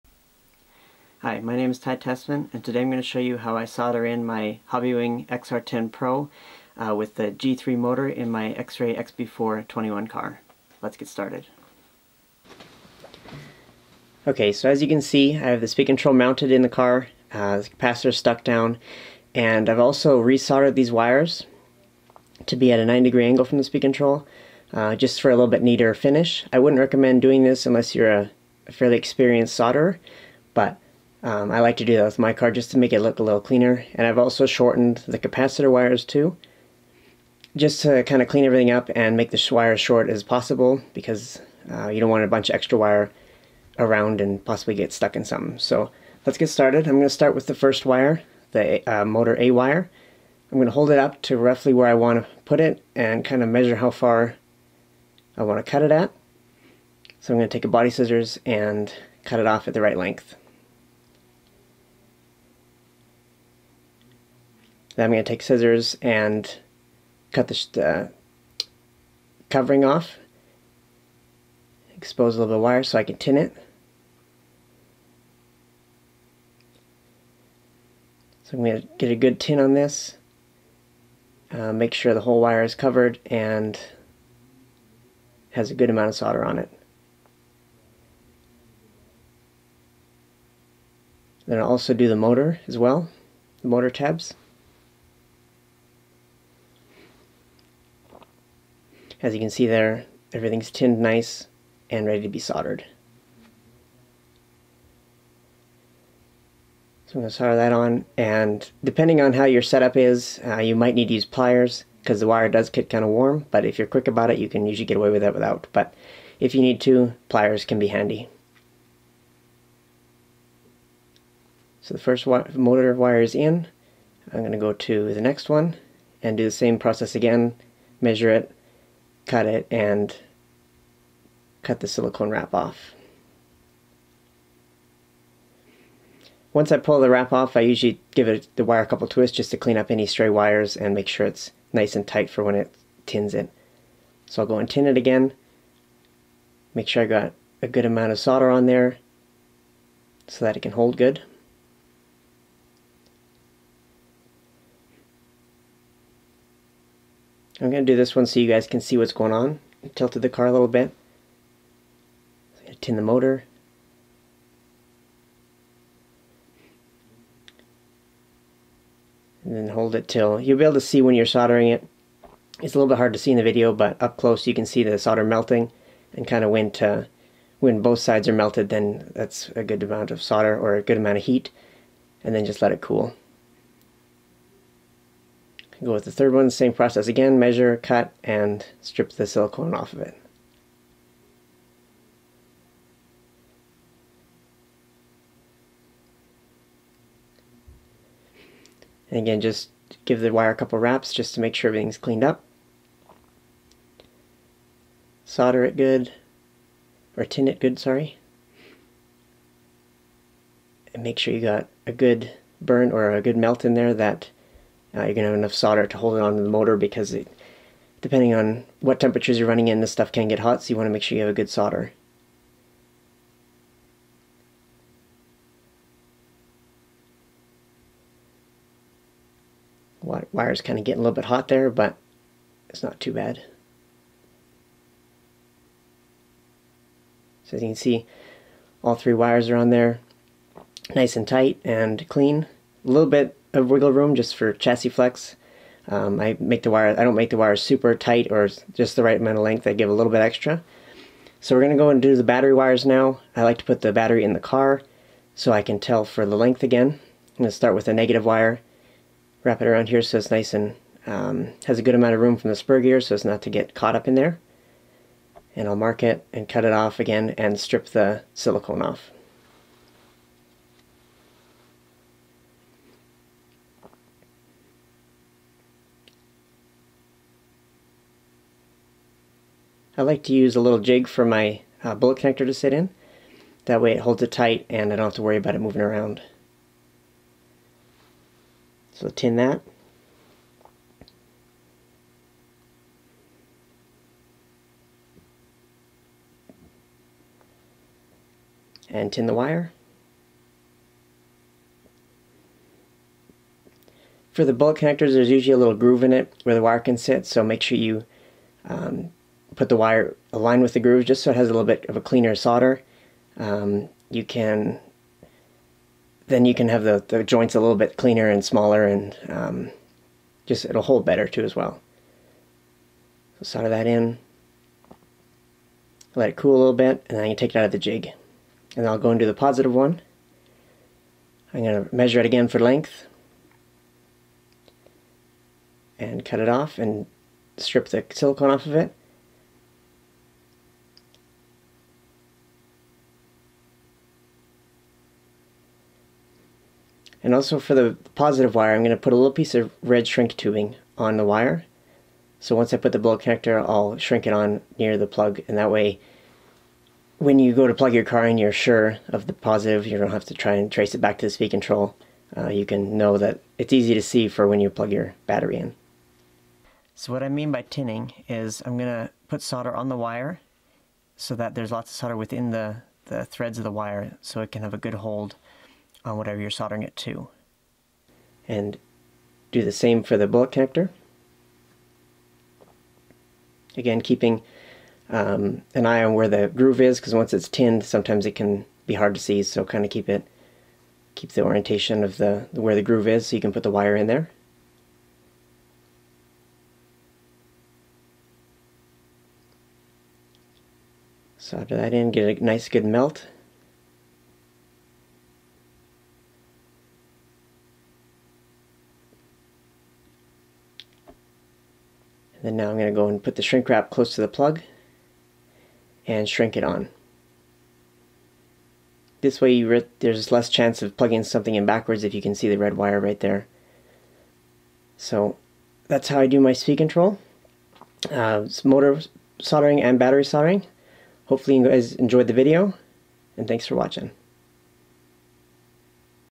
Ty Tessmann Pro Tip: Soldering your Electronics
Ty Tessmann shows you how to wire up your Xray XB4 with the Hobbywing XR10 Pro ESC and G2 motor.